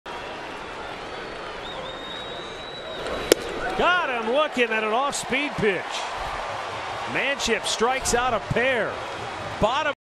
TOR@CLE: Manship tosses a perfect 9th inning
Jeff Manship strikes out a pair during a 1-2-3 9th inning, earning a victory in the Indians' comeback win over the Blue Jays